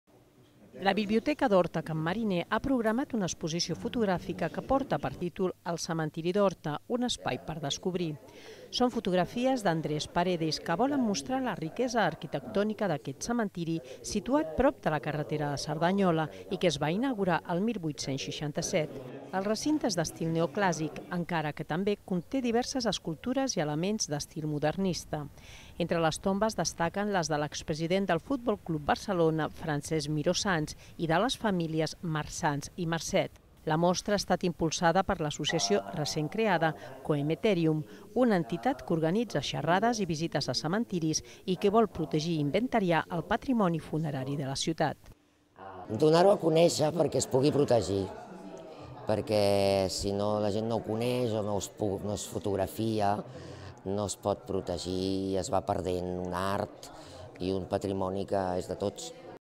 2017 03 24 Expo cementiri Horta
La biblioteca d’Horta-Can Mariner ha programat una exposició fotogràfica que porta per títol “El cementiri d’Horta, un espai per descobrir”. Són fotografies d’Andrés Paredes que volen mostrar la riquesa arquitectònica d’aquest cementiri situat prop de la carretera de Cerdanyola i que es va inaugurar el 1867.